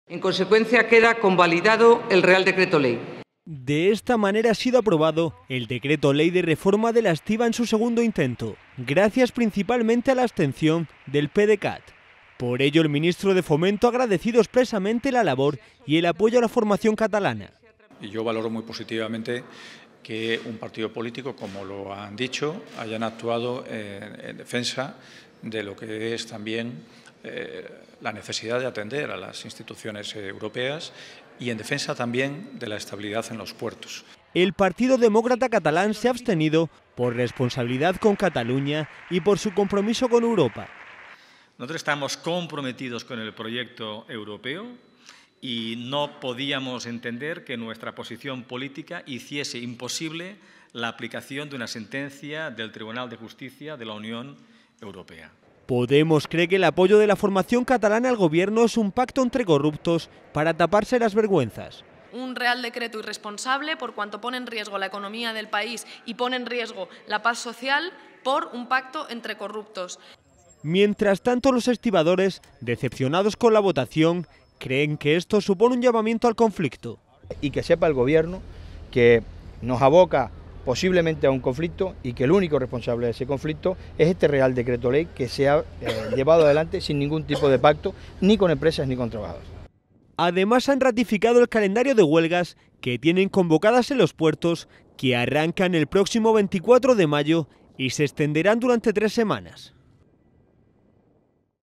El Congreso convalida la reforma de la estiba
De esta manera ha sido aprobado el Decreto Ley de reforma de la estiba en su segundo intento, gracias principalmente a la abstención del PDeCAT. Por ello el ministro de Fomento ha agradecido expresamente la labor y el apoyo a la formación catalana.